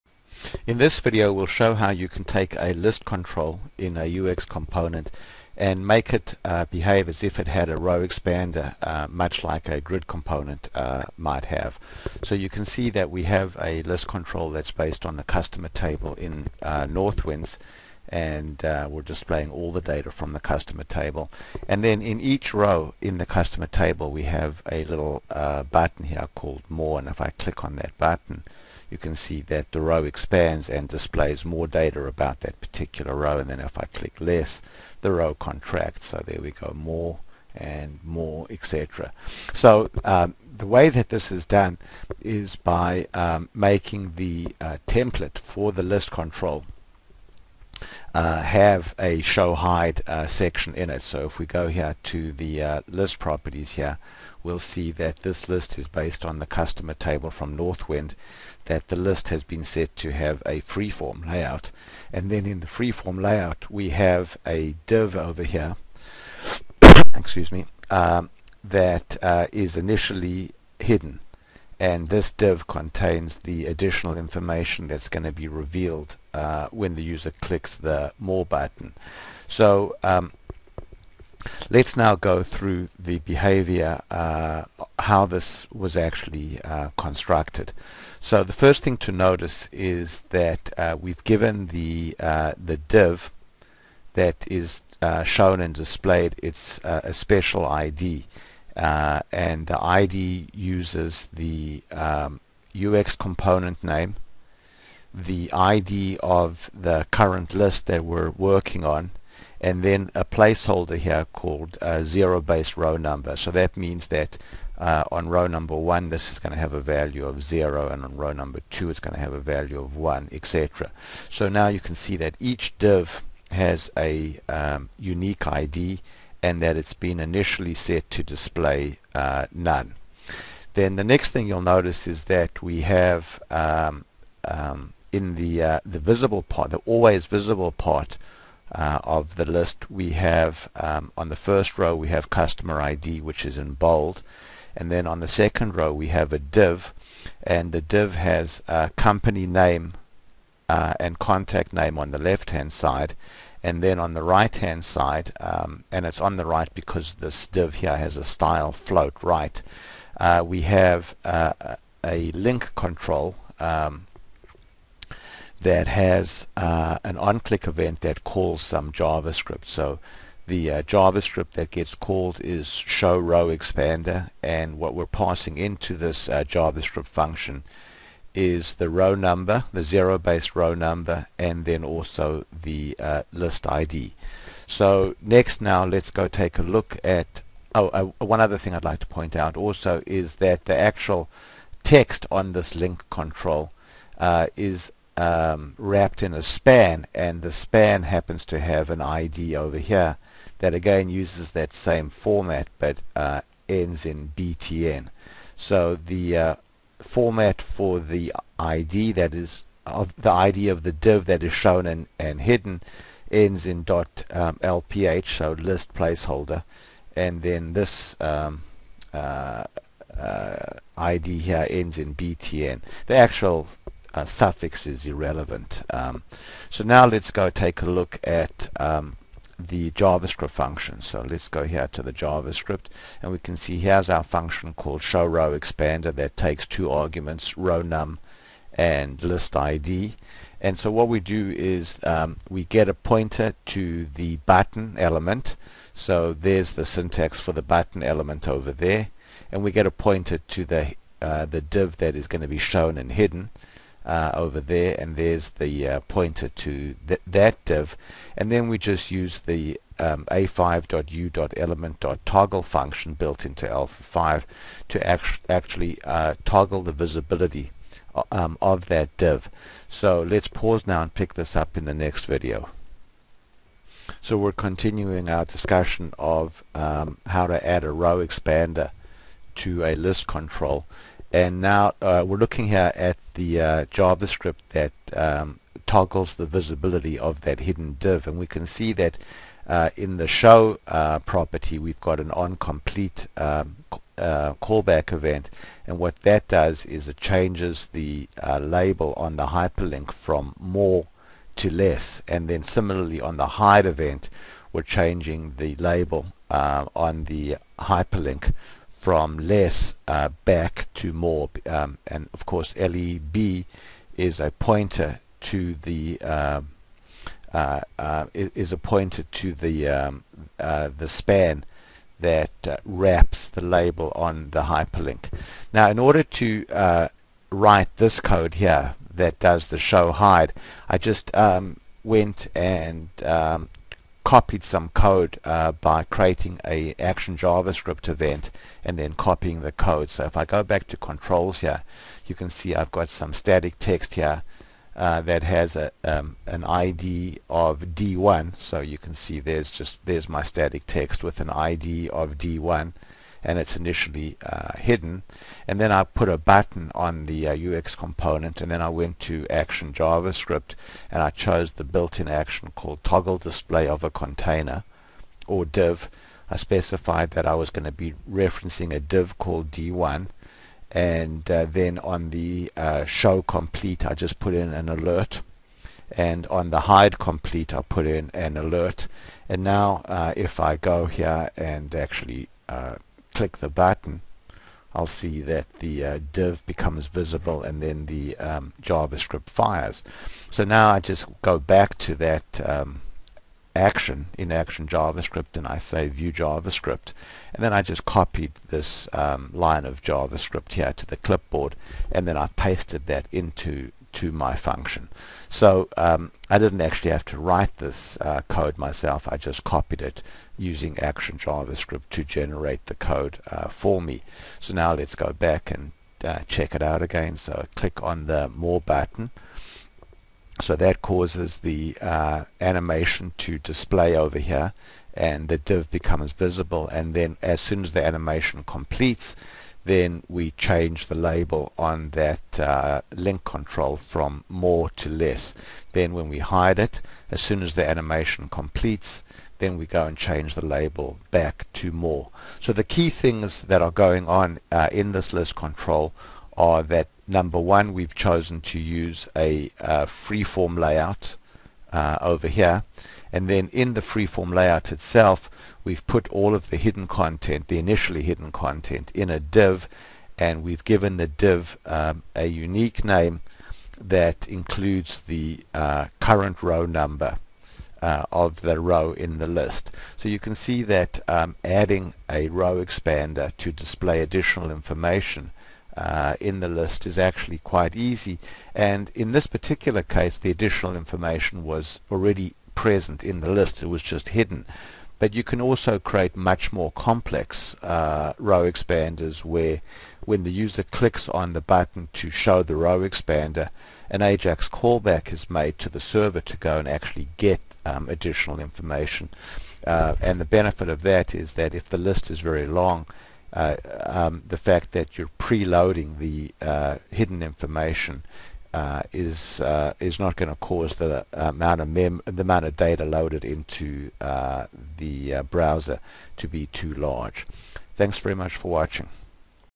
AA List Control - Adding a "Row Expander" to the List
UXL_V12--31 Alpha Five V12  UX Component simple row expander  Part 1 and 2

A common use case in a List is to be able to expand the current row to show additional information. The additional information can be pre-loaded so that when the row is expanded, no callback is necessary to get the additional data, or the additional data can be fetched from the server using an Ajax callback. In this video, we show how a "row expander" can be added to a List control to show some additional pre-loaded information for the current row.